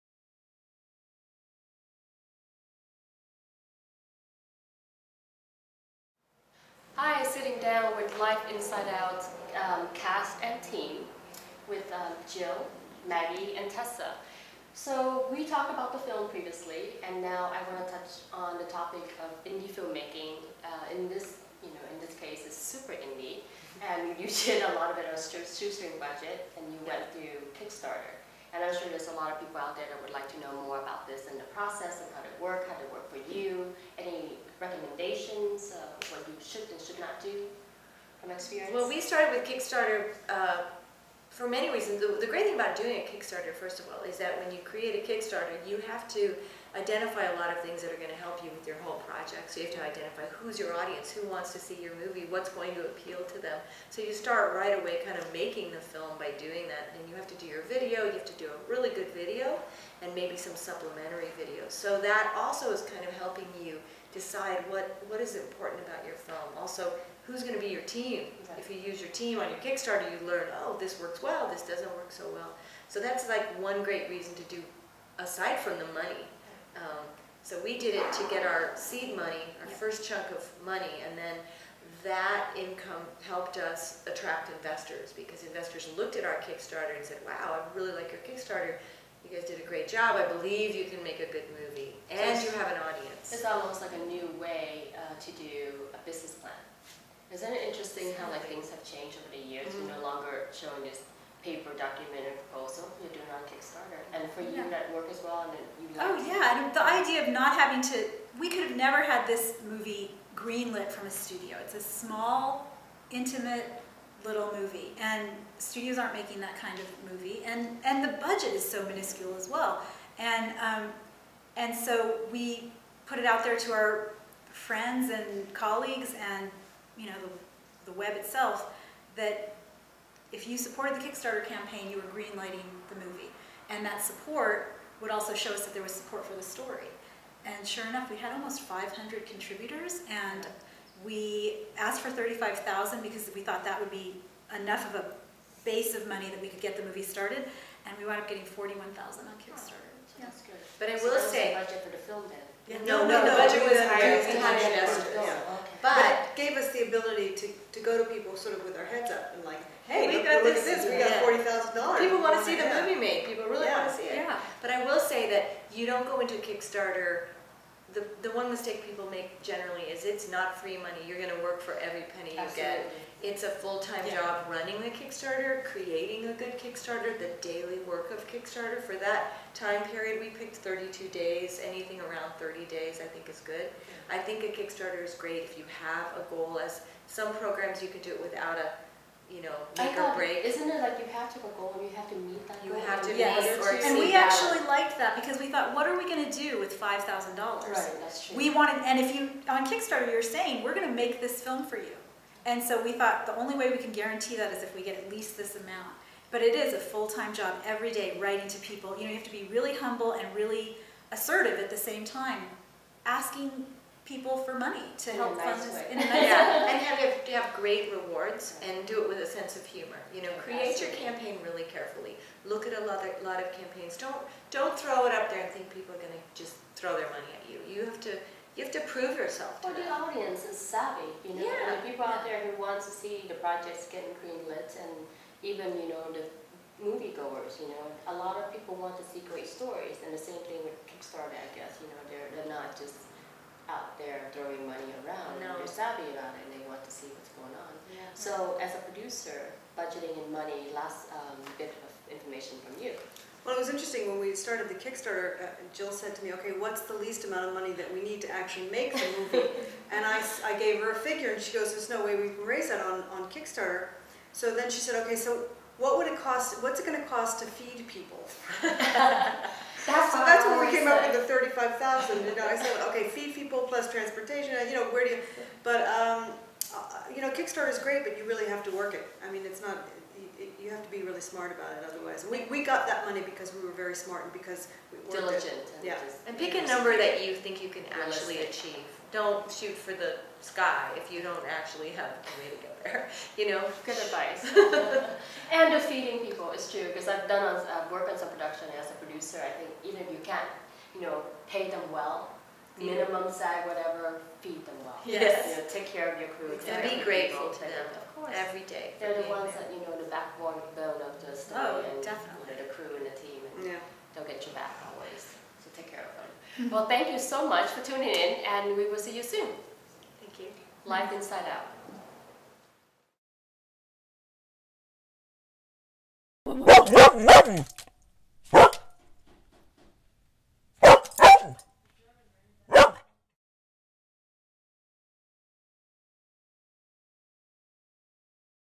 “Life Inside Out” movie and funding with Kickstarter with Sulinh Lafontaine
Insights to funding a film with Kickstarter for indie film “Life Inside Out” movie. Actress-Writer-Producer Maggie Baird, Producer Tessa Bell, and Director Jill D'Agnenica interviewed by Sulinh Lafontaine for MBN Newsvideoweb.